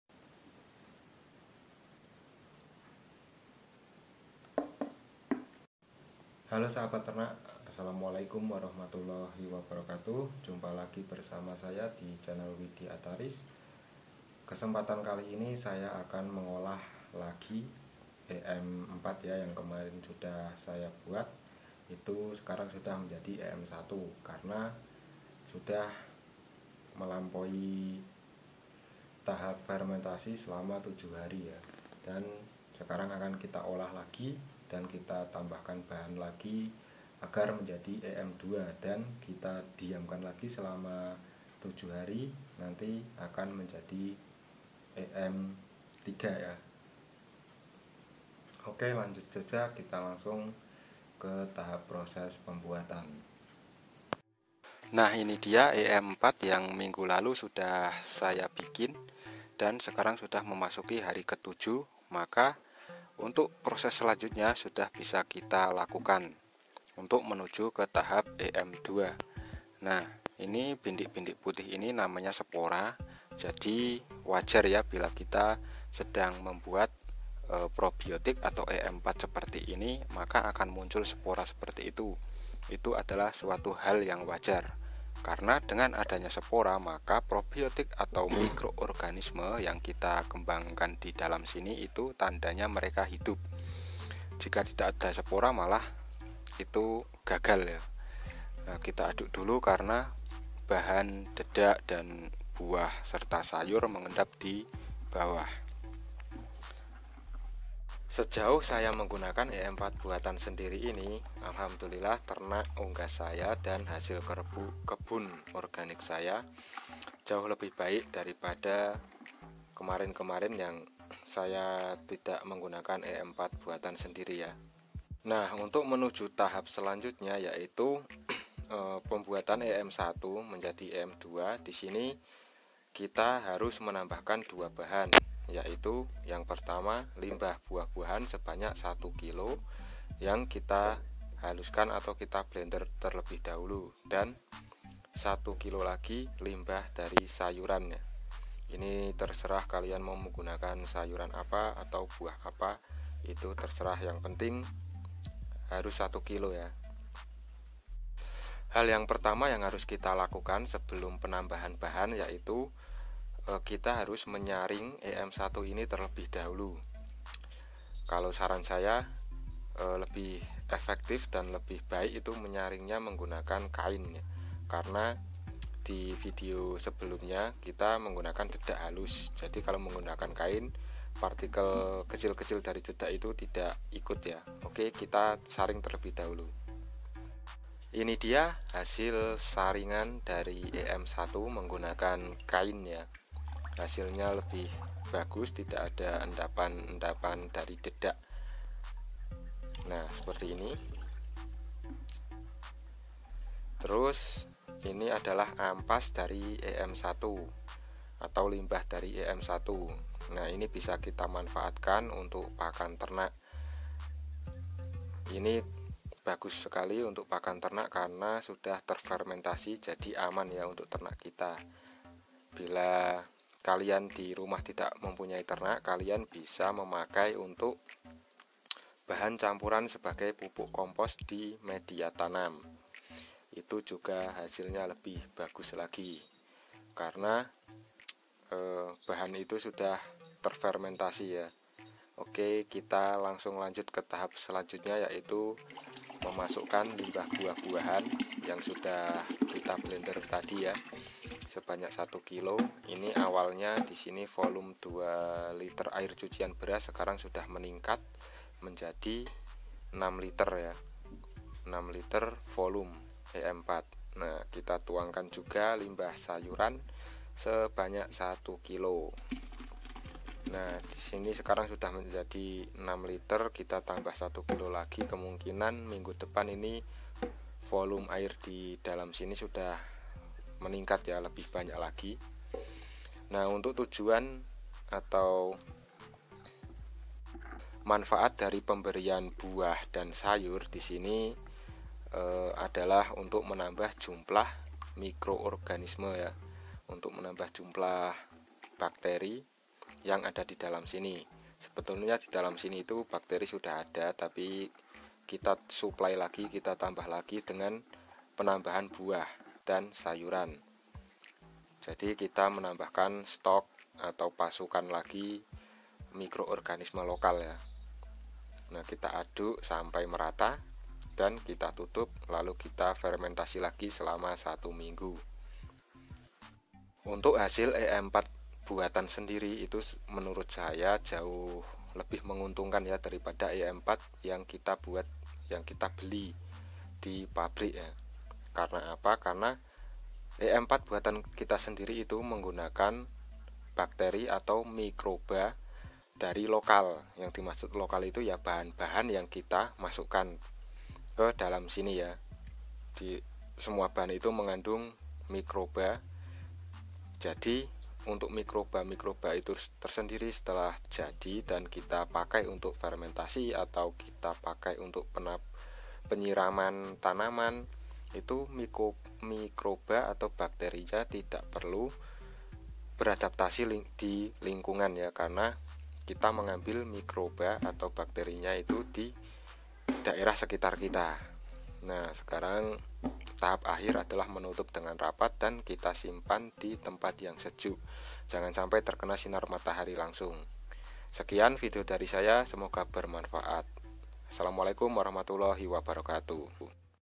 Cara Membuat EM4 [Part 2] Bahan Mudah & murah
Lanjutan dari video minggu lalu, video kali ini kami sampaikan proses pembuatan EM4 tahap ke-2, yaitu EM2.
Revisi:
Terima kasih atas saran dari mas Erwin Maulana agar deskripsi diberi rincian bahan-bahannya.
Terima kasih juga atas rangkuman dari mas Alex Sandi
Berikut revisinya:

* limbah sayur (diblender) 1 kg
* limbah buah (diblender) 1 kg
* air cucian beras 2 liter
* gula merah  1 ons
* bekatul 0.5 kg

1- campur semua bahan menjadi satu ke dalam satu wadah
2- tutup rapat
3- diamkan selama satu minggu didalam suhu ruangan
4- dihari ke-7, maka dinamakan EM1

proses ke EM2

5- dihari ke-8 saring EM1
6- tambahkan bahan: limbah buah 1kg (diblender) & limbah sayur 1kg (diblender) tanpa disaring
7- diamkan lagi selama satu minggu di dalam suhu ruangan
8- dihari ke-14, maka dinamakan EM2

proses ke EM3

7- dihari ke-15 saring EM2
8- tambahkan bahan: gula merah 1 ons dengan cara dilarutkan & bekatul 0.5 kg
9- aduk hingga rata
10- kemudian tutup rapat lagi selama 1 minggu
11. dihari ke-21, maka dinamakan EM3

Proses ke EM4
11. di hari ke-22 saring EM3
12. Biarkan EM3 di dalam wadah tertutup di suhu ruang selama 1 minggu
13. di hari ke-28, maka sudah menjadi EM4
14. Saring dan sudah bisa diaplikasikan, baik untuk kocor, semprot, maupun untuk fermentasi bahan organik lainnya.

EM4 bisa dibuat sendiri yaitu melalui beberapa tahap agar hasilnya lebih maksimal.